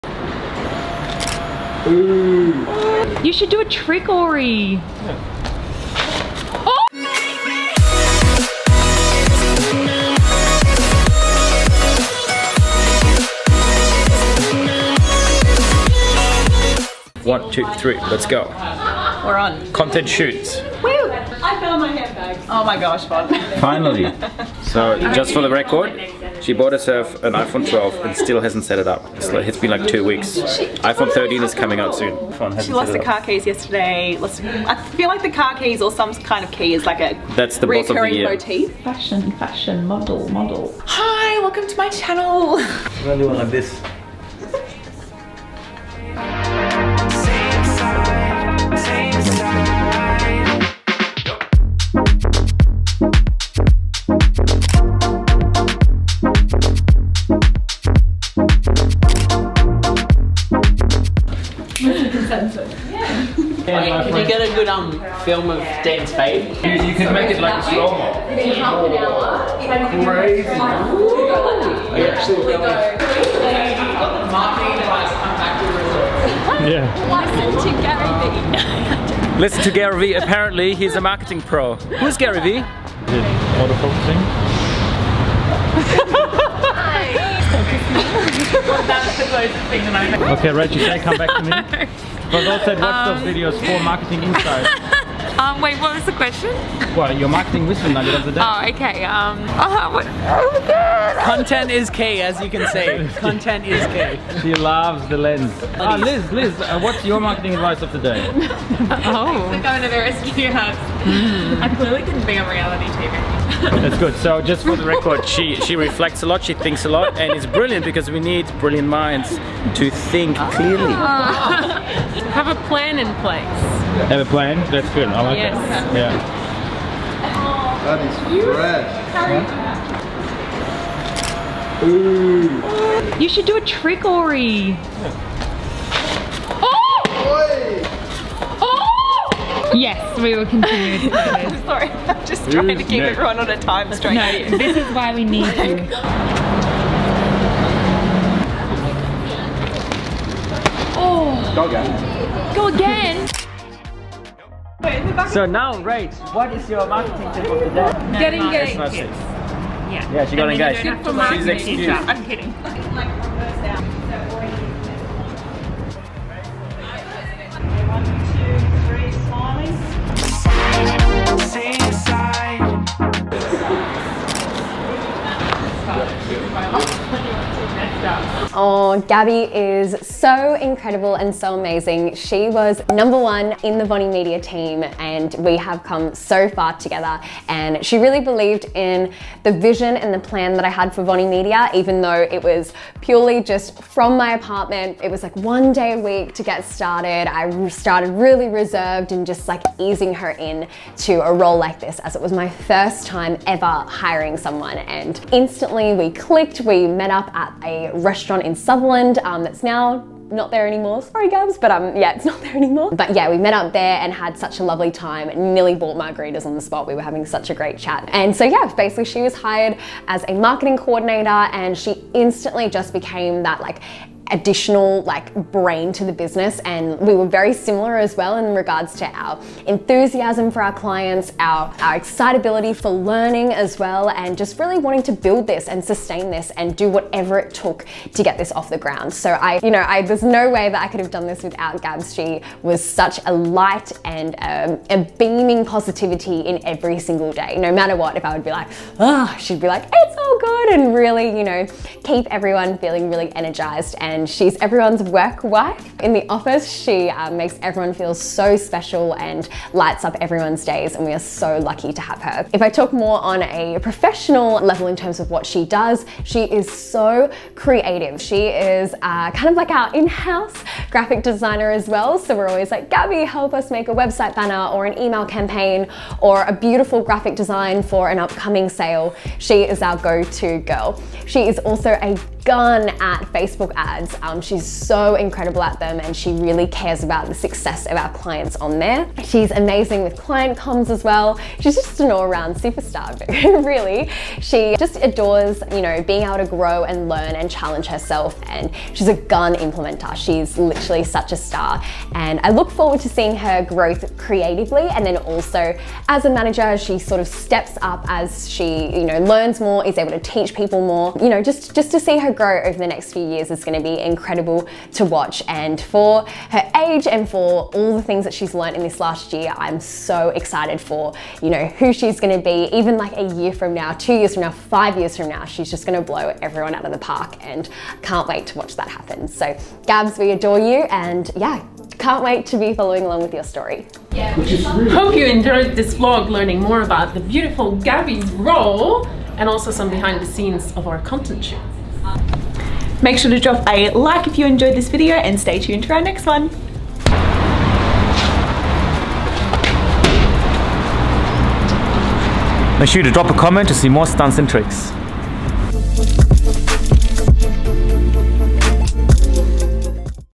Business Photography - Behind the Scenes of Portraits and Group Photos | VonniMedia
Come behind the scenes with the lovely team at VonniMedia and learn our process of taking corporate photos quick and easy. I also take you through Gabby’s role and how she really helped my skyrocket this business as she was the first team member alongside me!